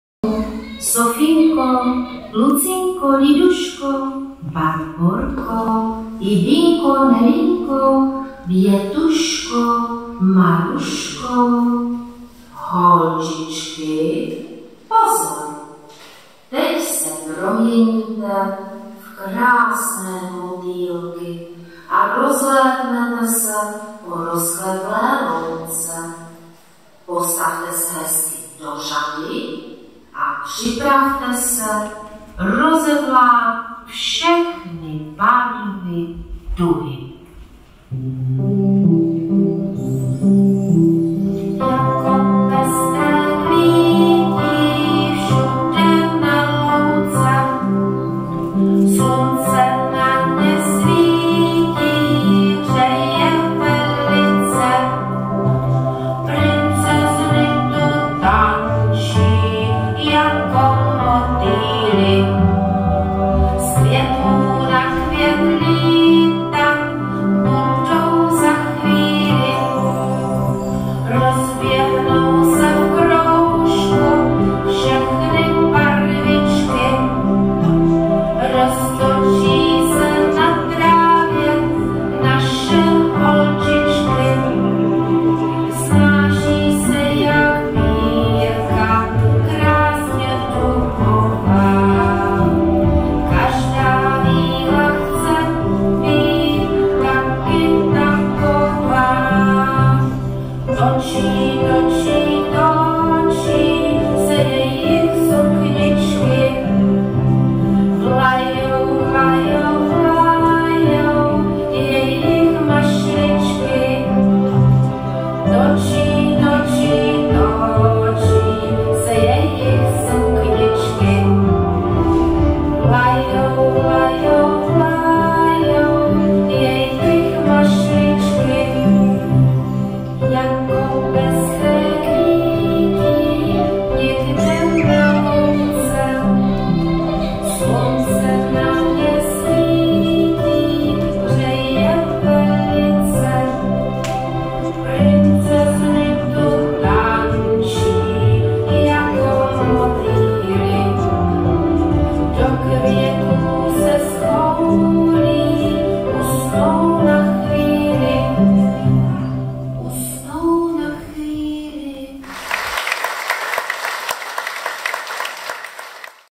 Vystoupení dětí z TS Mata Hari
Slavnostní otevření kulturního domu v Ostravě - Hrabové, 1.7.2022
video : Ilona a Libor Hromádkovi